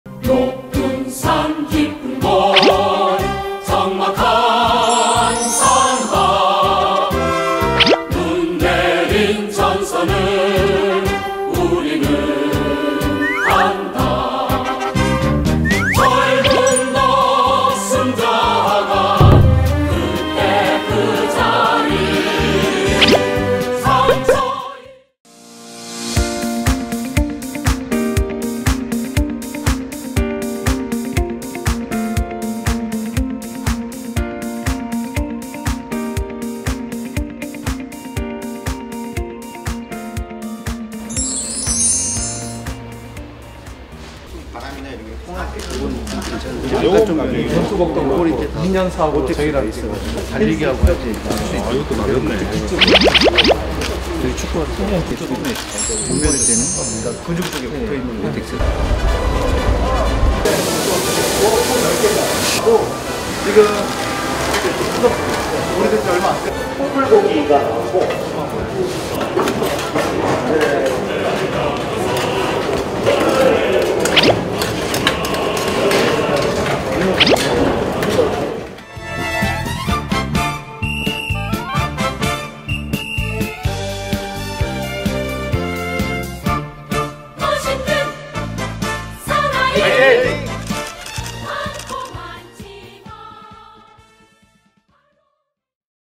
장병이 만족하고 부모 걱정을 더는 국방물자 조달혁신, 계속하겠습니다!
이종욱 조달청장 육군훈련소-육군군수사령부 방문
왜???!!!😮
조달청이 조달하는 군수품 현장점검~😊
조달청은 군수품 중 급식류🍱와 피복류🥾를 공급하고 있습니다.
2020년 급식 및 피복류 등의 군수품 조달업무를 방사청으로부터 이관받아
지난해 2조 8,391억 원의 급식·피복류 등의 군수품을 공급했습니다.
획일화된 군수품 공급을 벗어나 ‘국민 눈높이에 맞는’ 다양하고 품질좋은 군수품 공급에 최선을 다하겠습니다.👍